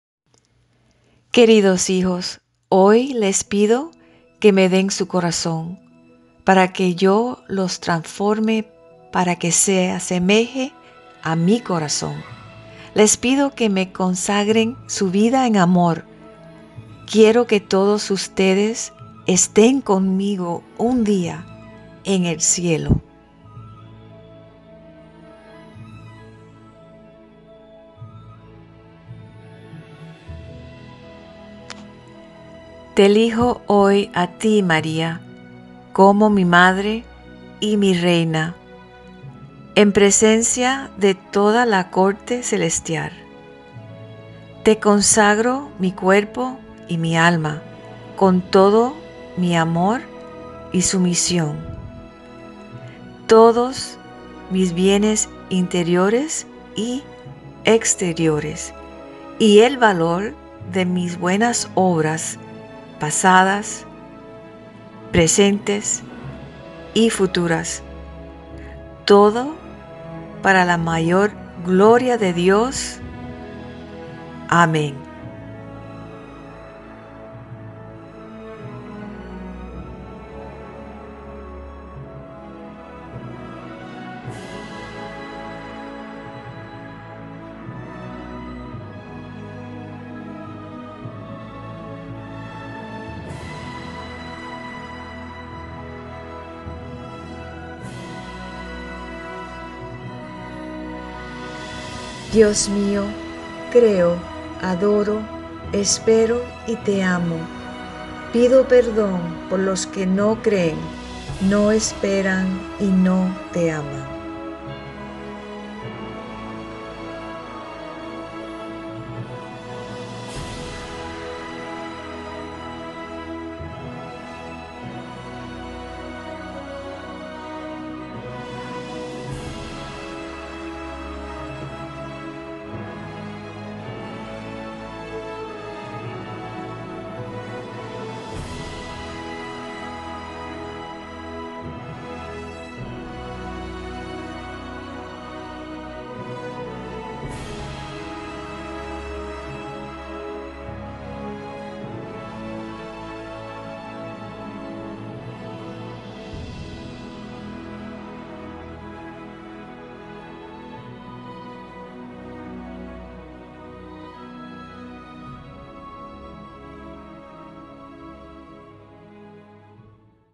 Consagración a Maria para los Niños Día 9
Mi oración es que con esta Consagración de 9 días,  niños podrán acercarse más a Jesus y Maria.  En los tiempos que vivimos ahora, necesitamos más que nunca la oración.  St. Luis de Monfort tenía una devoción muy grande de traer a todos a Jesús por las manos de su Madre Maria.